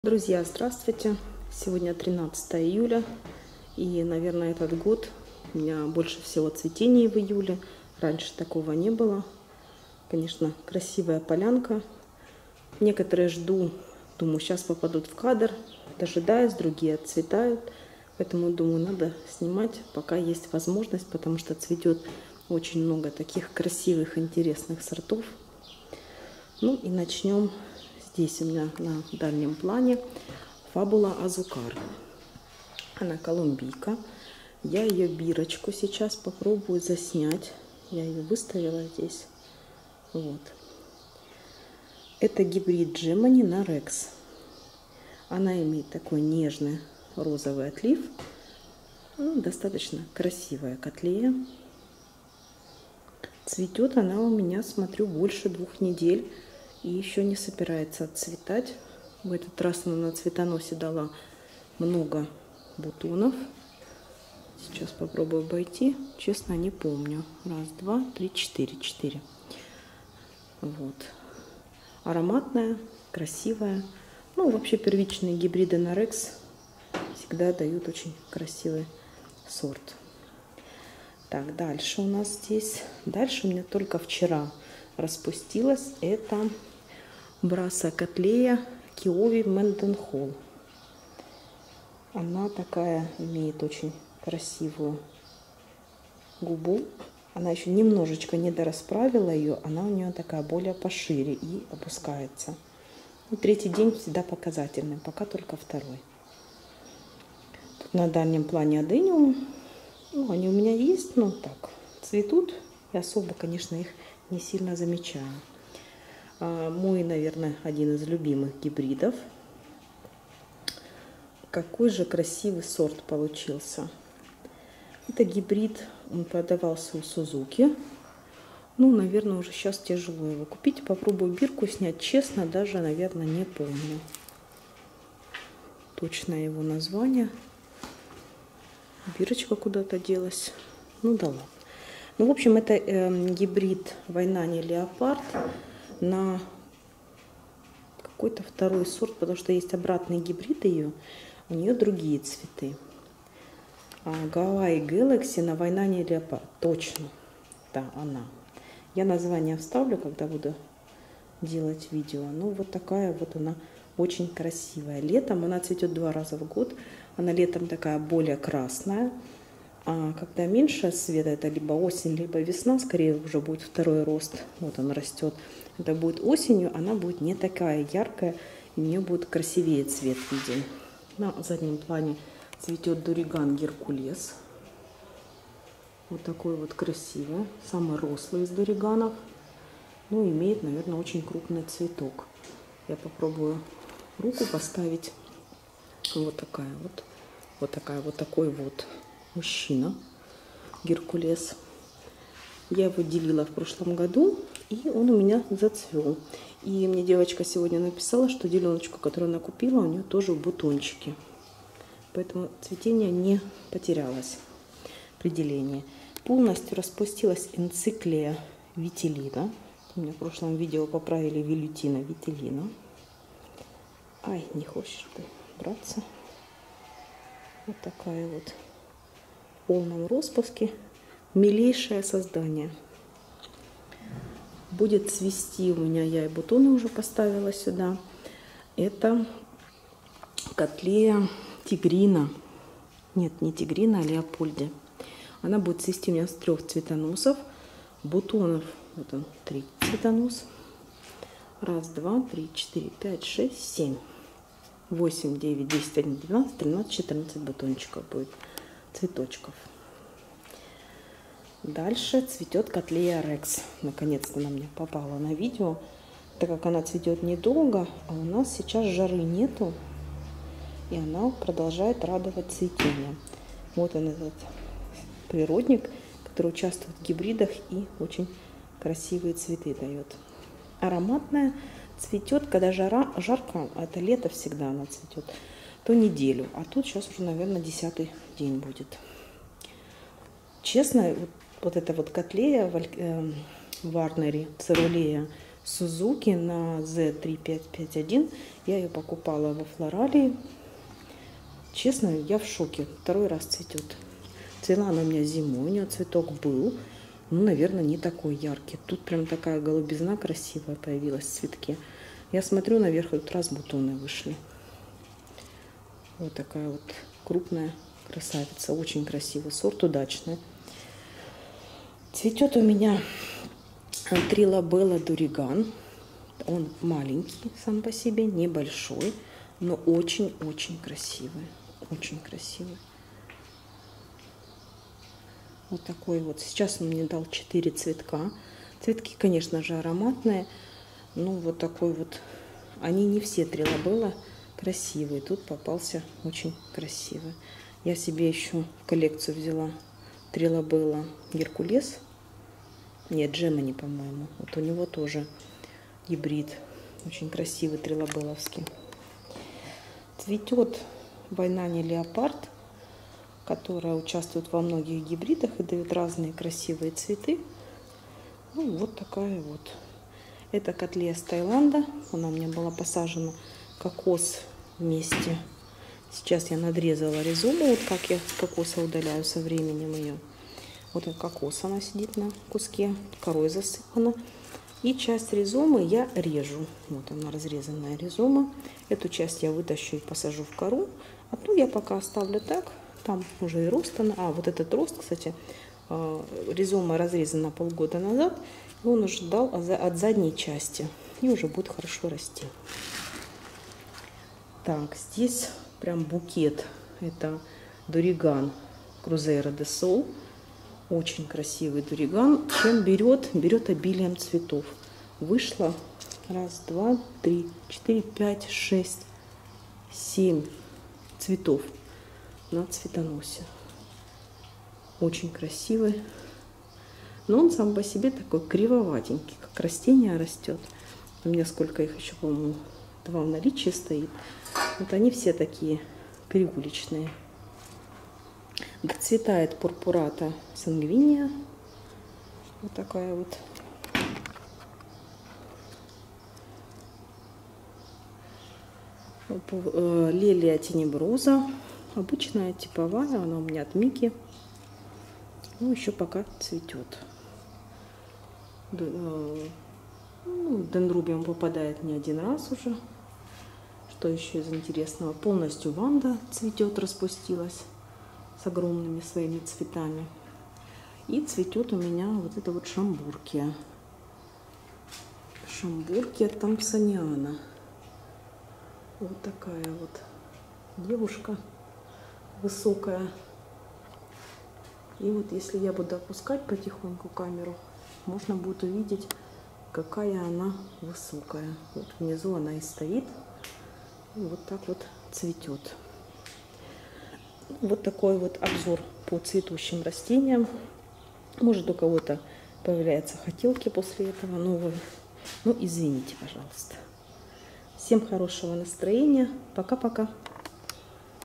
Плоды июля 2023: Cattleya rex , Rlc. Durigan Tetraploid ,Cattleya Evas Fabula y Azucar и др.
На видео :
- Rlc. Durigan Tetraploid ( Rlc. Durigan "Perseus" x Rlc. Durigan "Gemini")
- Cattleya warneri coerulea ('Suzuki' x 'Z-351')
- Cattleya rex
- Blc. Durigan Hercules
- Blc. Durigan Cruzeiro do Sul
- Rhyncholaeliocattleya Waianae Leopard.
- Laelia tenebrosa
- Cattleya Evas Fabula y Azucar (jenmanii SA x rex)
- Laelia purpurata var. sanguinea
- Encyclia vitellina
- Schomburgkia thomsoniana
- Blc. Hawaiian Galaxy x Blc. Waianae Leopard
 - Bc. Keowee 'Mendenhall'
 - Blc. Waianae Leopard x Cattleya guttata ( бутоны)
- Cattleya leopoldii ( бутоны)